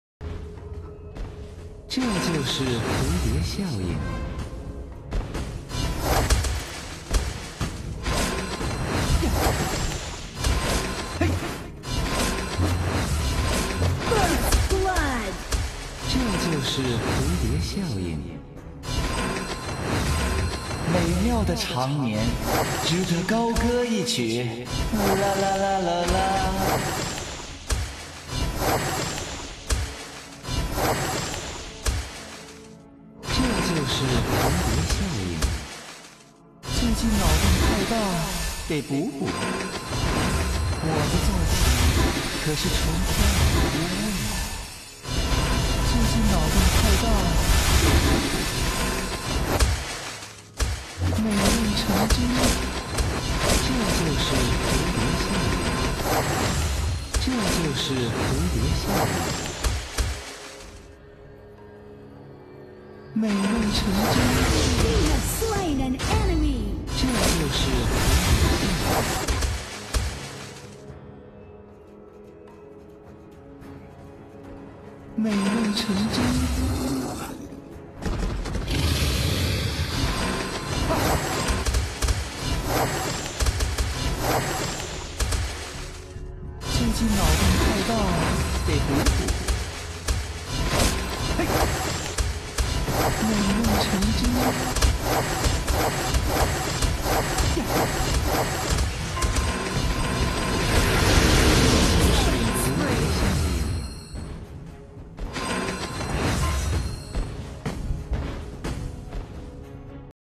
庄周特效重做！真的太美了！质量超高~小翅膀好可爱~
这个特效是目前我最喜欢的~感觉很有品质感~